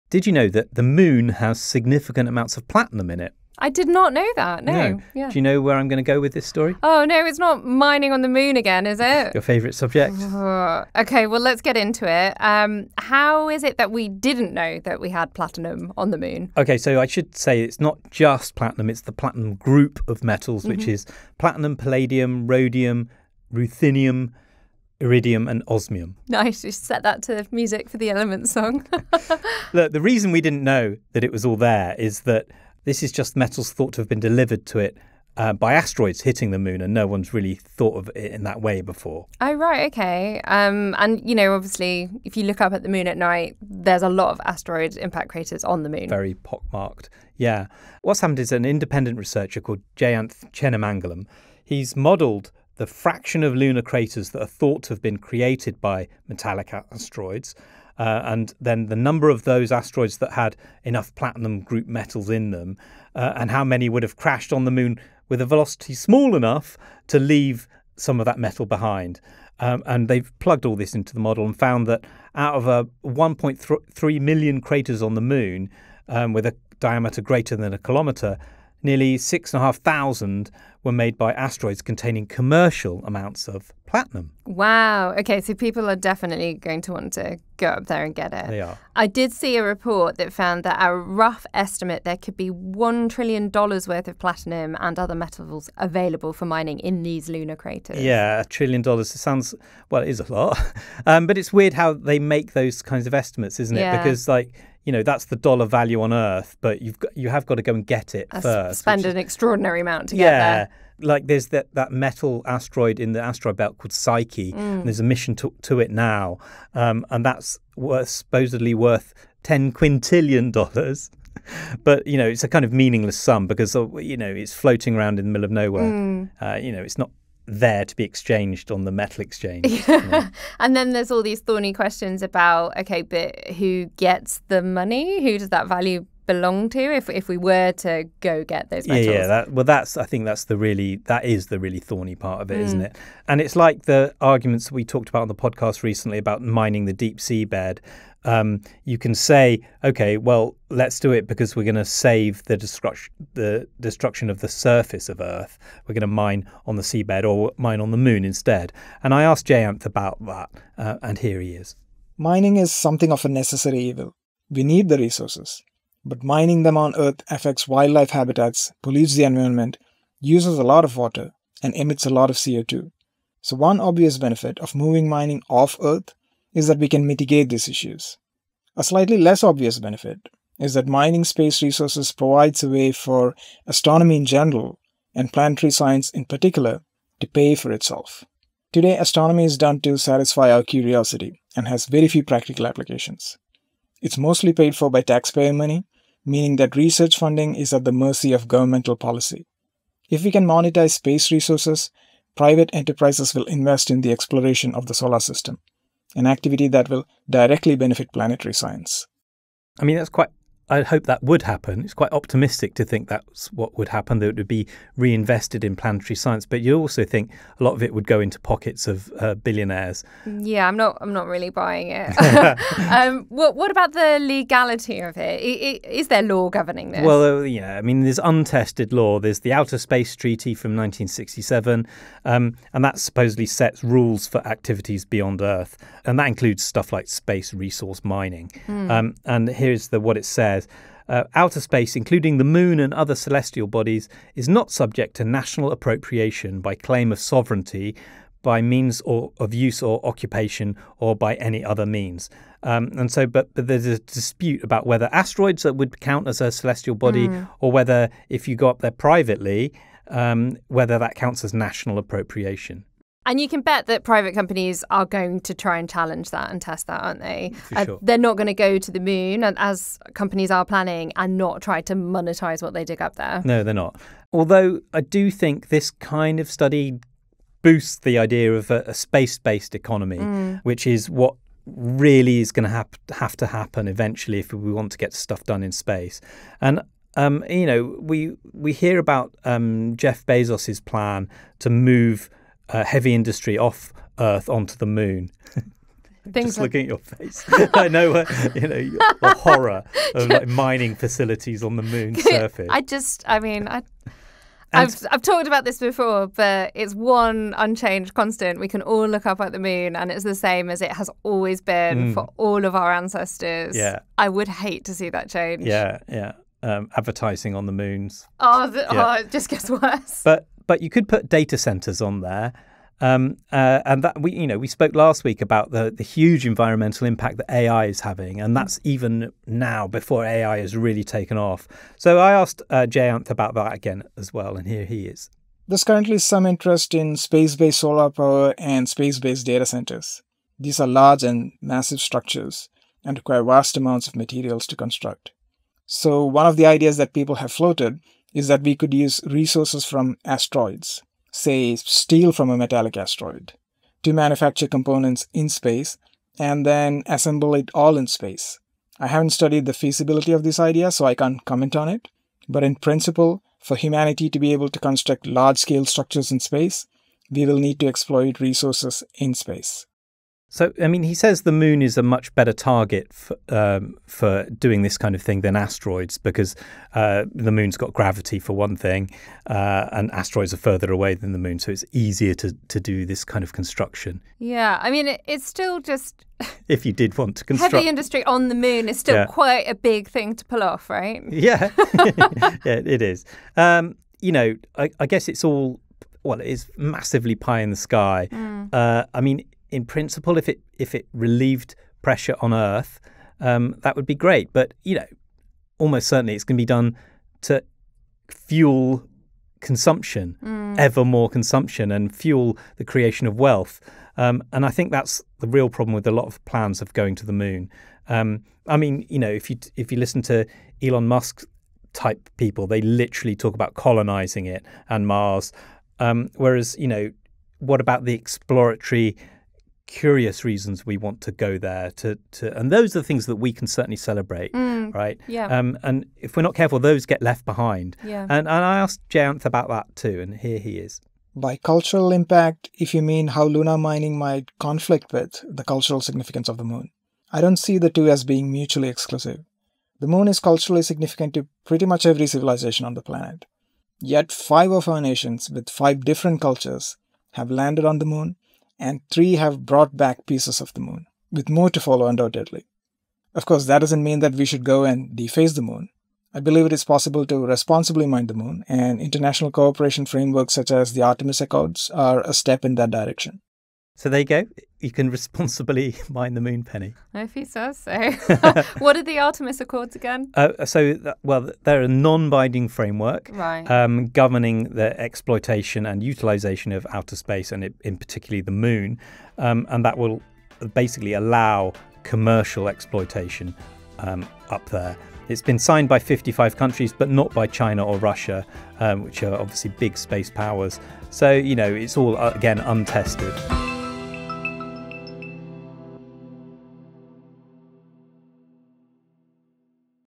There’s $1 trillion of platinum on the moon - who will mine it first?
It’s been discovered that the moon is probably home to $1 trillion worth of platinum. Researchers suggest nearly 6500 of the Moon’s craters were made by asteroids containing commercial quantities of platinum, palladium, rhodium, ruthenium, iridium and osmium. Does that mean space companies are going to head out on mining missions imminently…and should they? Hear the reasons for and against mining on the Moon.

Hosted by Rowan Hooper and Penny Sarchet, with guest Jayanth Chennamangalam.